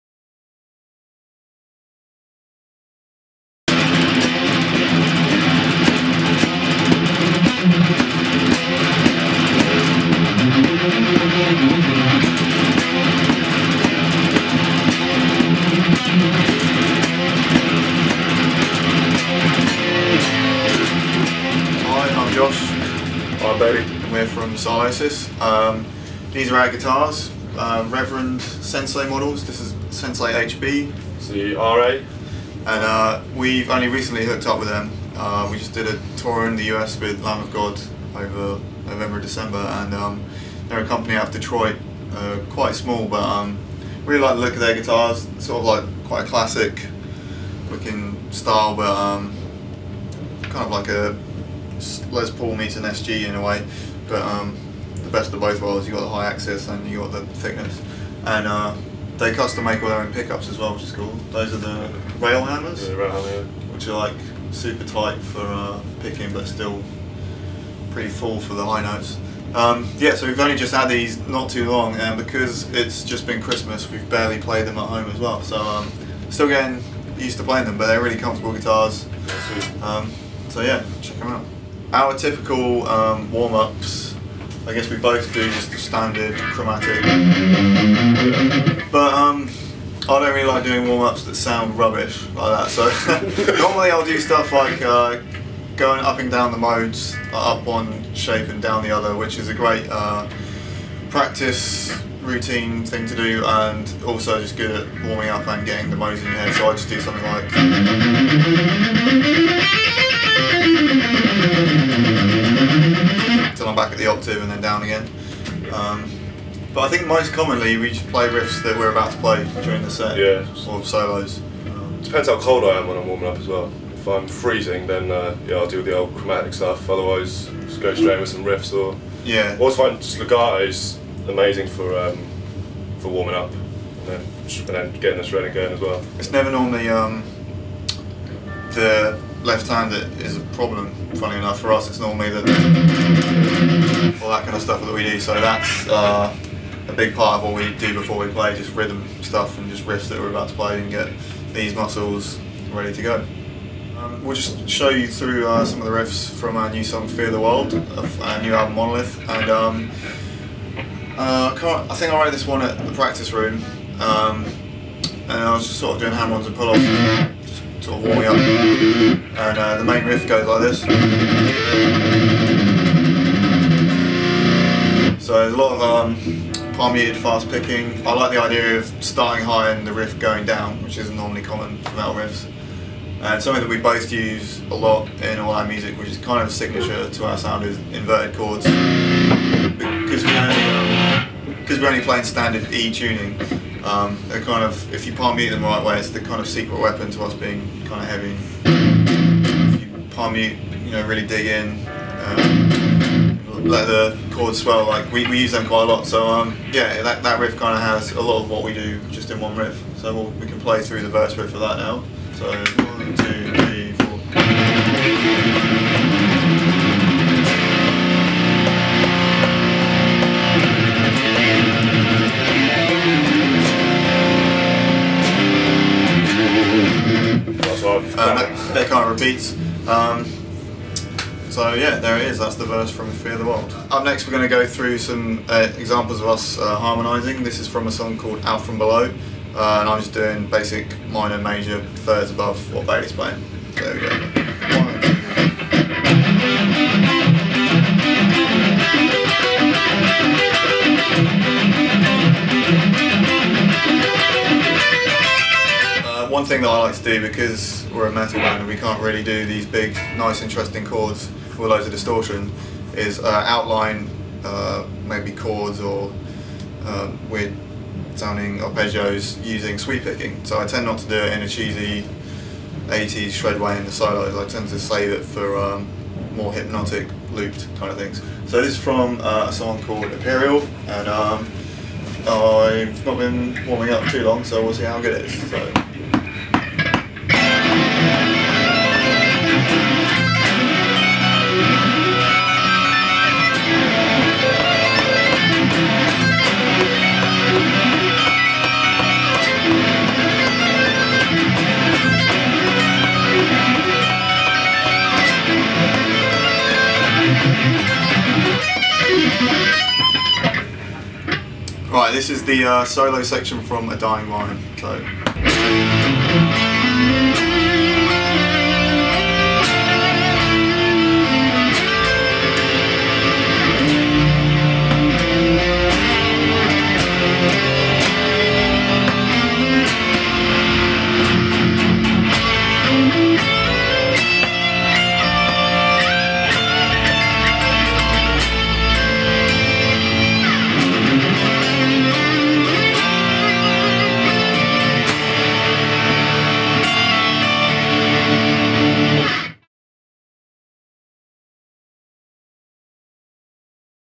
TG239: Sylosis talk new Reverend guitars, demonstrate rhythm and lead techniques
Josh and Alex from Sylosis talk new Reverend axes and demonstrate their impressive rhythm and lead skills.